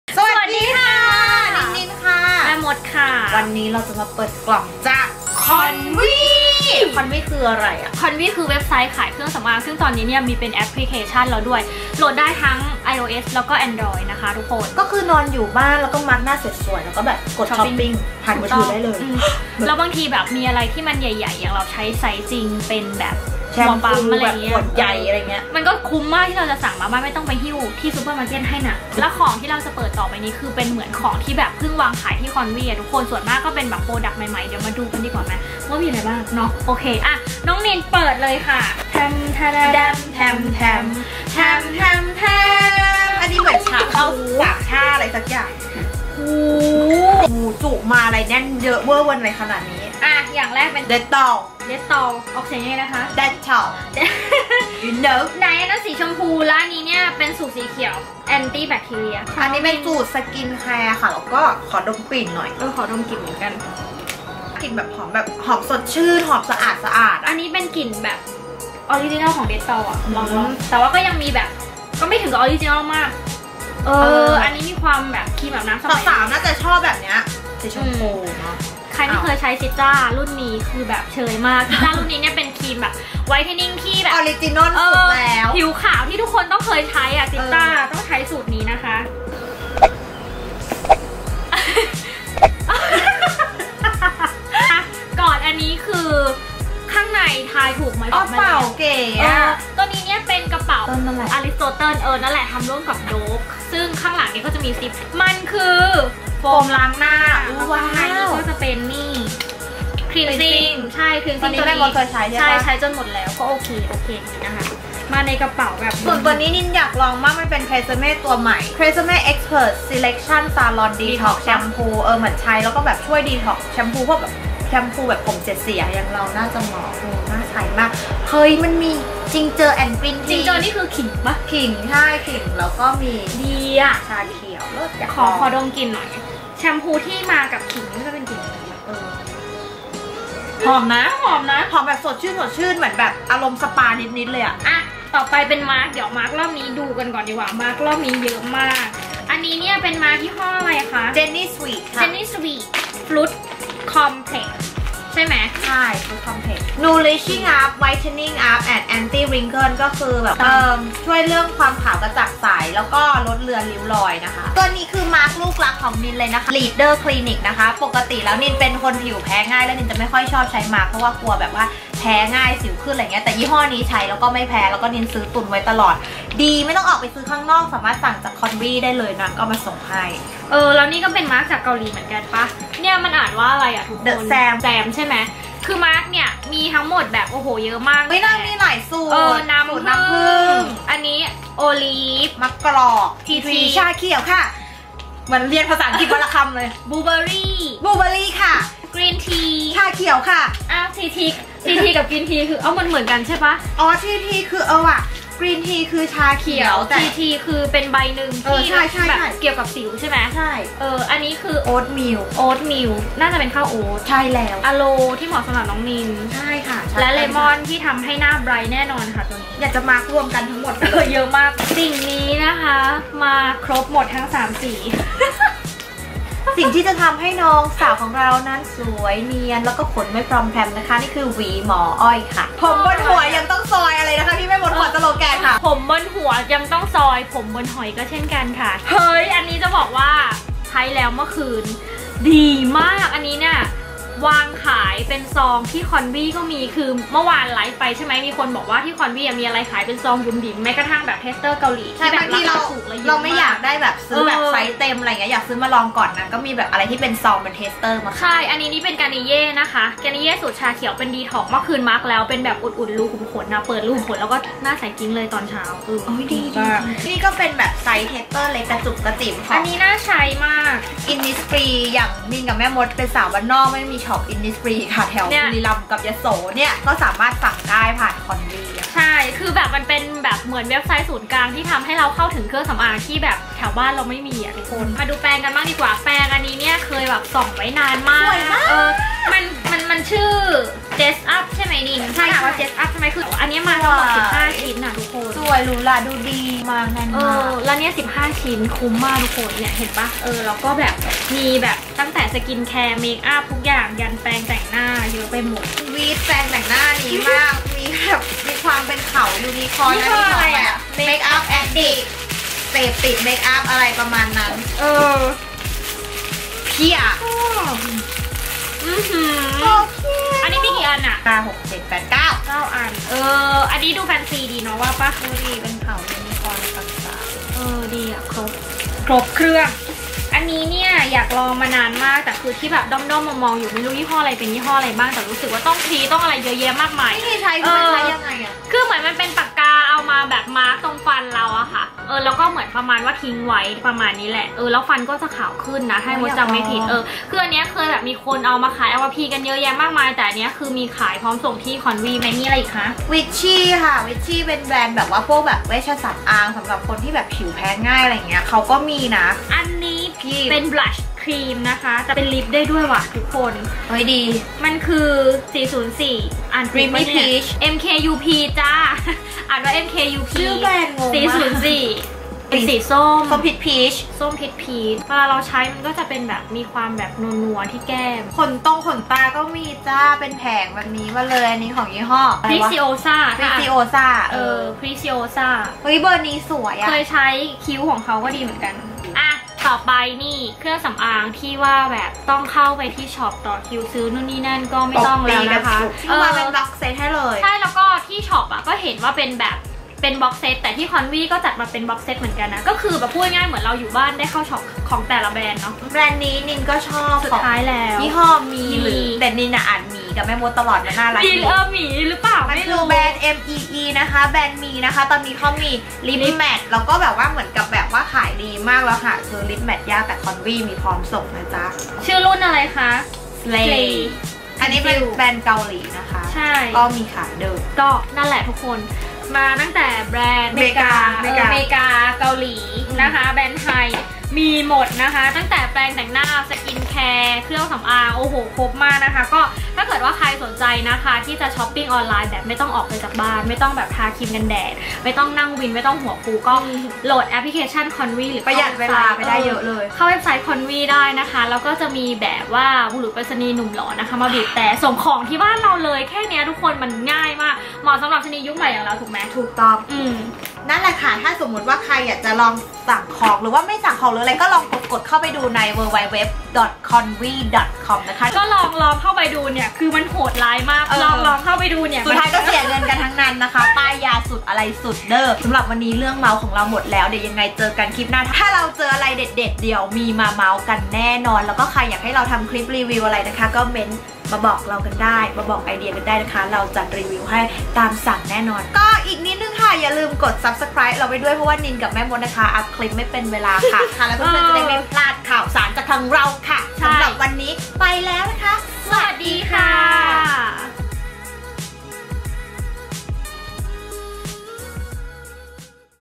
BEAUTY HUAL l #2 เปิดกล่อง konvy ช้อปปิ้งเครื่องสำอางค์ออนไลน์
เปิดกล่อง Konvy มาดูว่าในกล่องมีอะไรเด็ดๆบ้าง
ชะนีอย่างเรานี่ อยู่ไหนก็ช้อปได้ นอนหลบแดดอยู่บ้านก็เสียตัง !!
แล้วนี่สะดวกเวอร์ โหลด App มานอนส่อง กดสั่งแบบกรุบๆ
นี่ตอนนี้มี 11.11 Mega Sale ลดกระหน่ำสูงสุดถึง 92%
วันนี้ -14 พ.ย เปิด App ส่องของหน่อยเป็นไง
ไม่เสียตังนอนไม่หลับเด้อ !